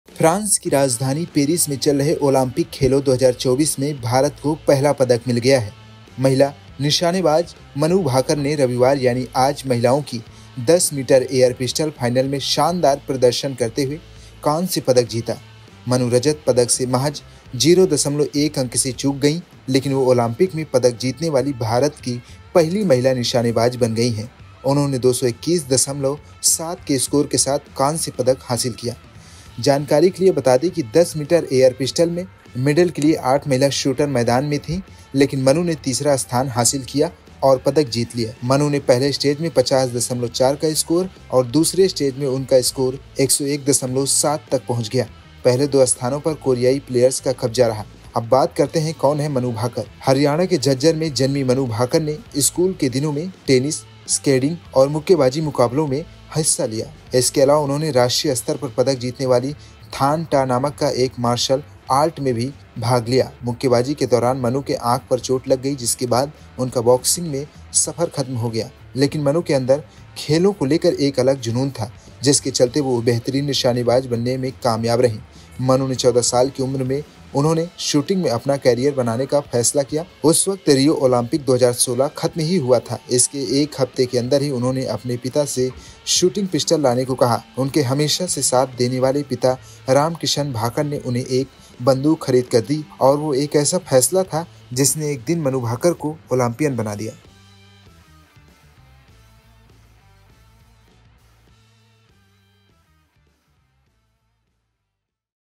Manu Bhaker Paris Olympics 2024: शूंटिग में पदक जीतने वाली पहली महिला भारतीय खिलाड़ी बनी
Manu Bhaker Paris Olympics 2024: मनु भाकर ने शूंटिग में पदक जीत के इतिहास रच दिया है. वो ओलंपिक इतिहास में शूंटिग में भारत के लिए पदक जीतने वाली पहली महिला खिलाड़ी बन गई है.